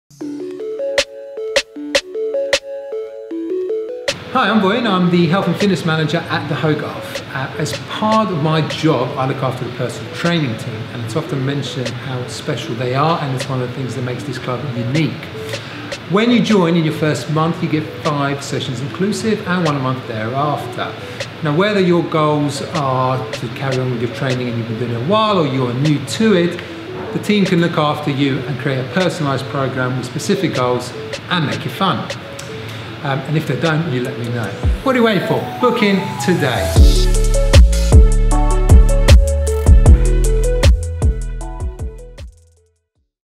Personal Training at The Hogarth Club in Chiswick
Vojin Soskic, Health & Fitness Manager encourages you to book in your Complimentary Personal Training sessions at The Hogarth Health Club in Chiswick, West London.

Receive 5 FREE sessions in your first month and one free per month thereafter.

Enquire today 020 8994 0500.